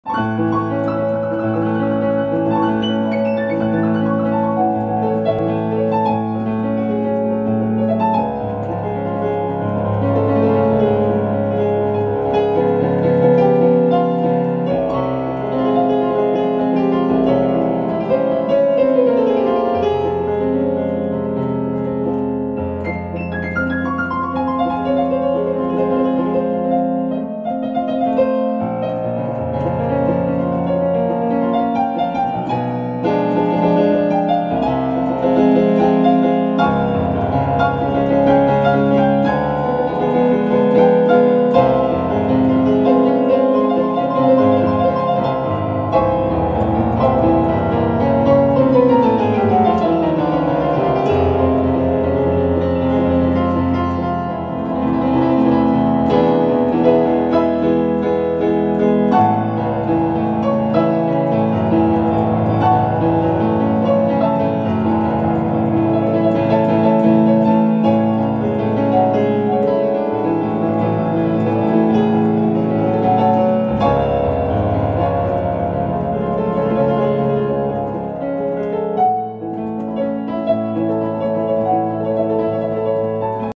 Improvisación después de una noche sin dormir. Piano
Improvisación piano. Perdón por lla errores, pero no he dormido.